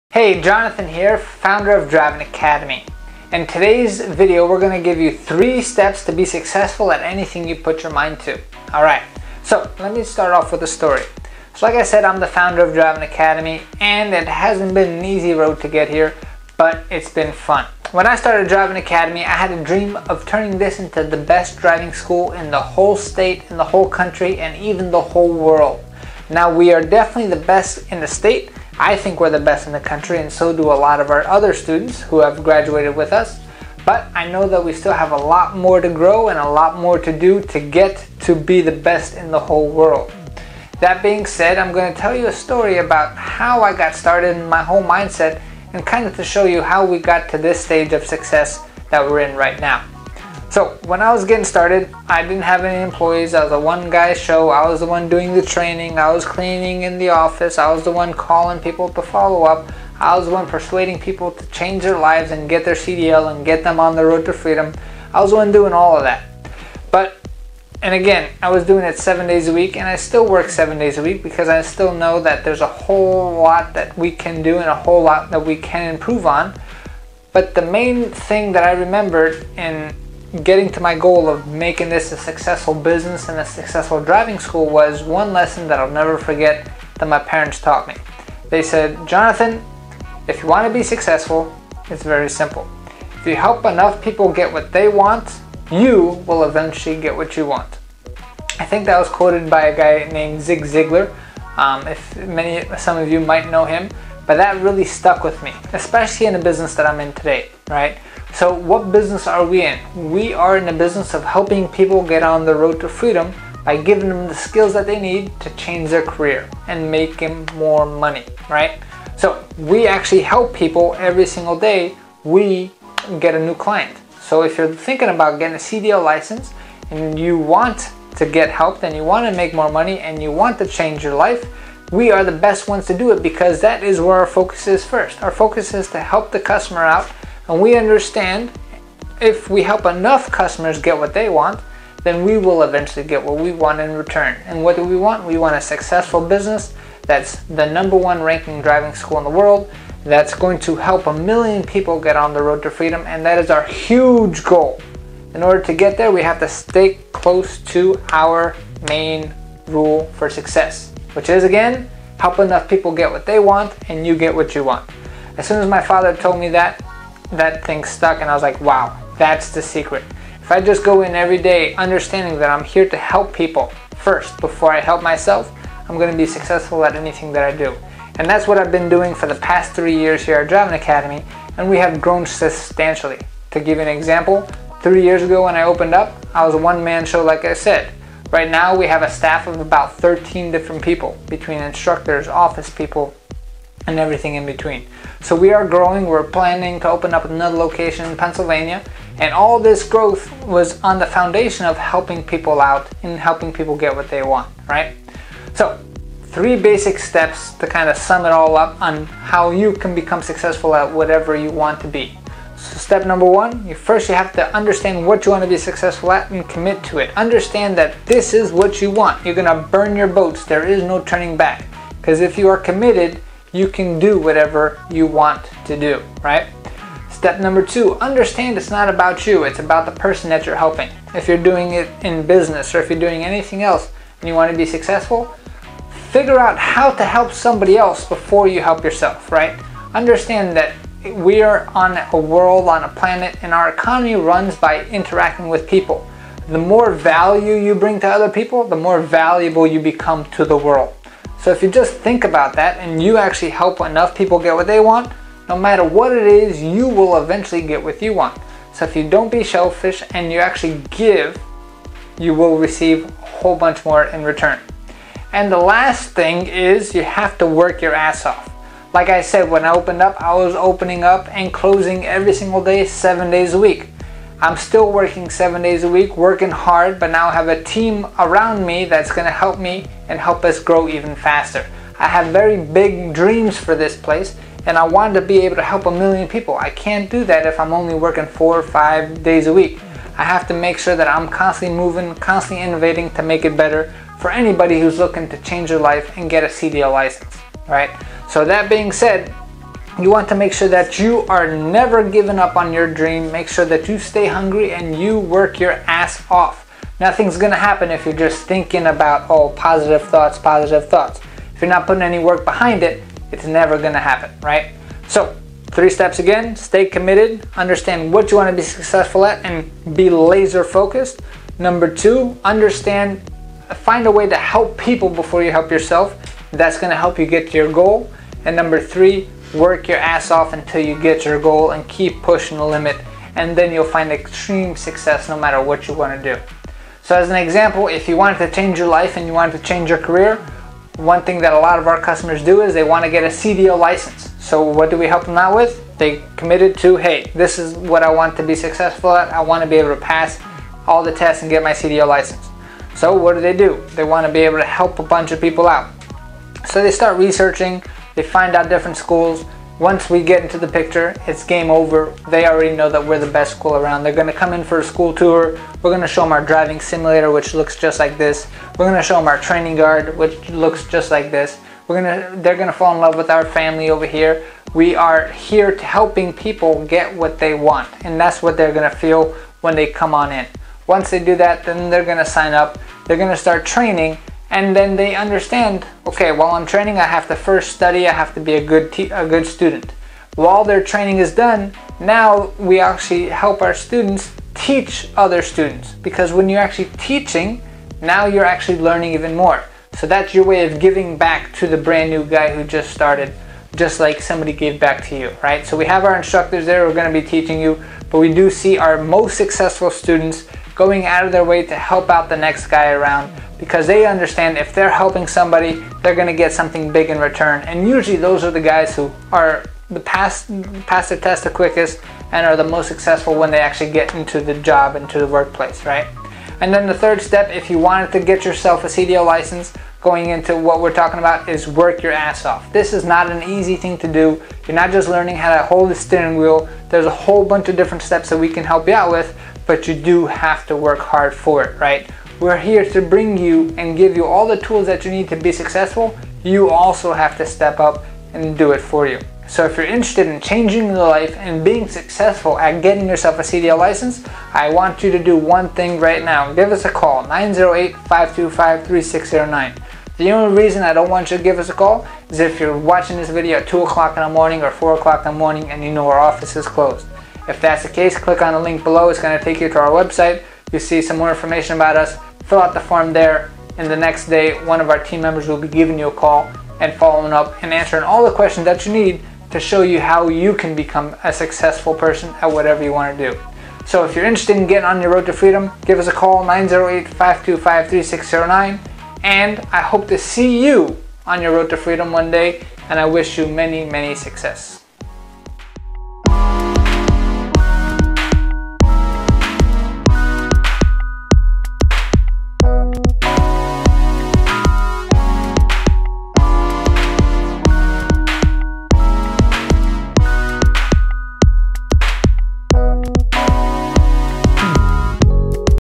How to Succeed at ANYTHING you Put Your Mind to - CDL Driving Academy
Driving Academy

We can succeed at anything we try - the success part is in YOUR hands! Jonathan is here to tell you how you can make your goals a reality - including getting a commercial driver's license!

If you're looking to get your CDL and want to train with only the BEST, give us a call TODAY to get started!

200 East Edgar Road, Linden NJ